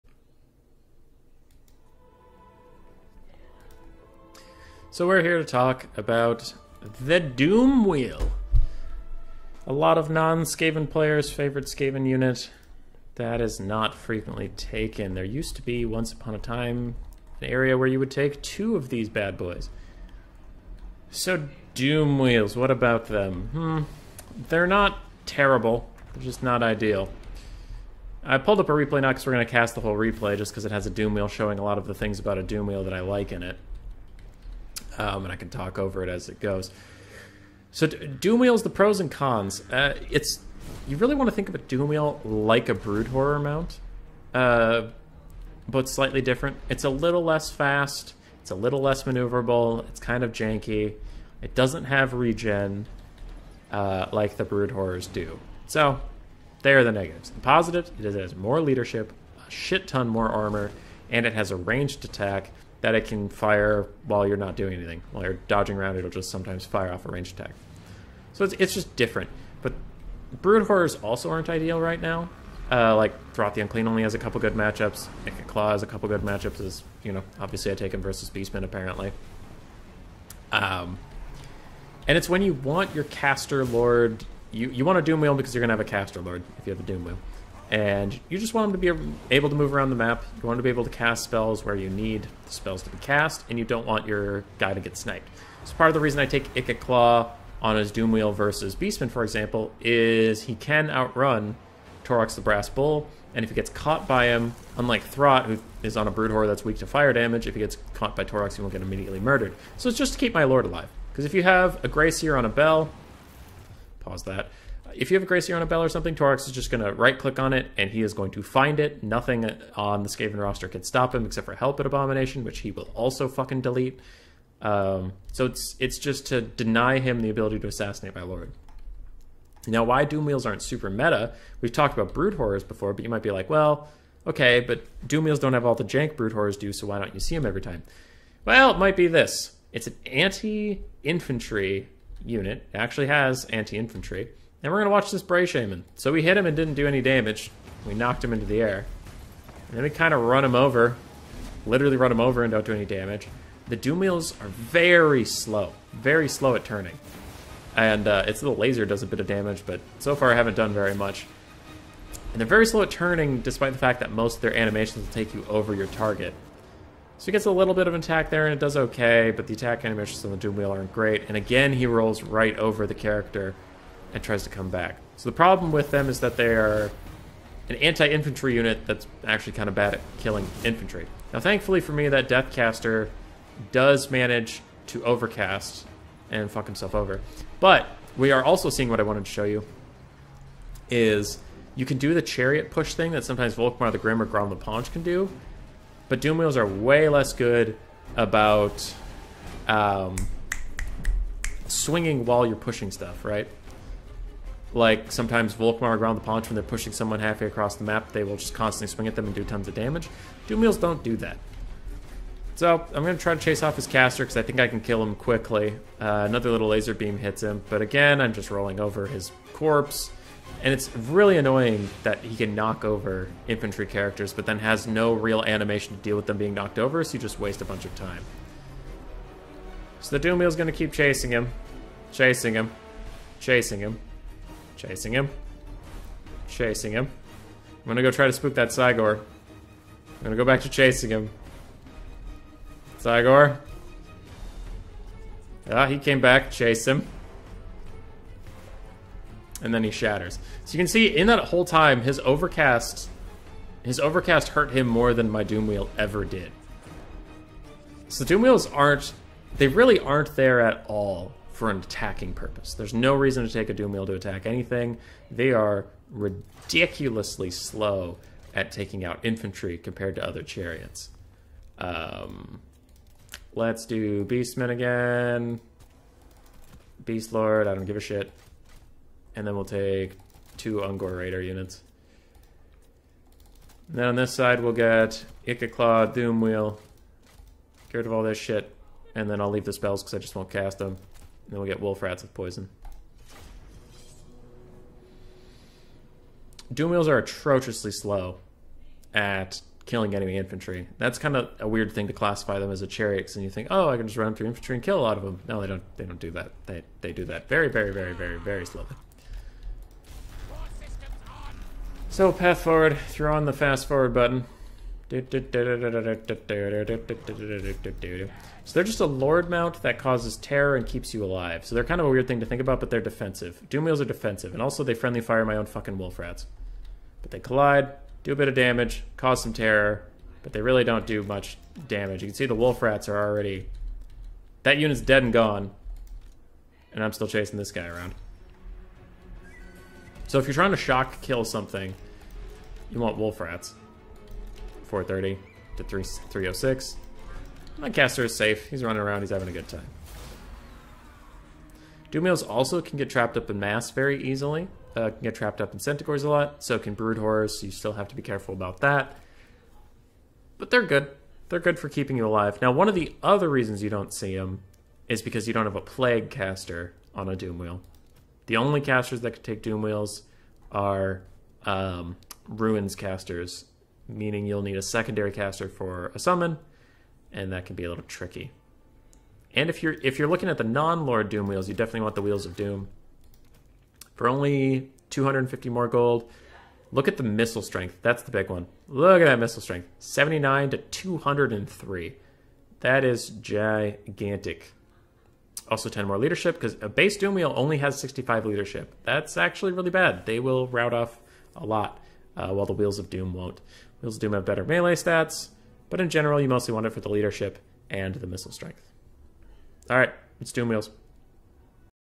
Doomwheels, why and what and how and why.
Doomwheels aren't great, but if you need your lord in an armored baby cage to not die die they work ok.